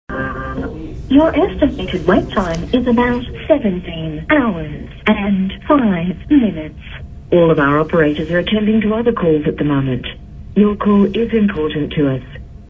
Small businesses on hold for 17 hours.
Battling small business operators and their staff have been waiting more than six weeks for promised financial support from the Andrews Labor Government.

It only gets worse, with the waiting time on the support hot line stretching beyond 17 hours.

No amount of hold music can lessen that pain.

A disgraceful way for Labor to treat struggling small business operators and their hard working staff.